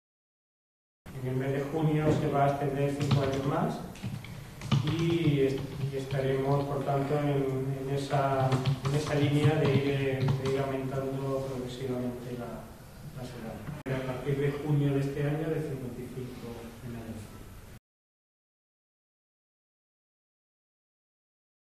El programa de detección de cáncer de colon se aplicará desde los 55 años
La Ciudad ampliará el programa de detección de cáncer de colon desde los 55 años. Así lo ha confirmado el portavoz del Ejecutivo, Alberto Gaitán durante la rueda de prensa posterior al consejo de Gobierno, donde ha anunciado que será a partir de este verano cuando se amplíe la edad para realizar estas pruebas, que actualmente se dirigen a personas de entre 60 y 69 años.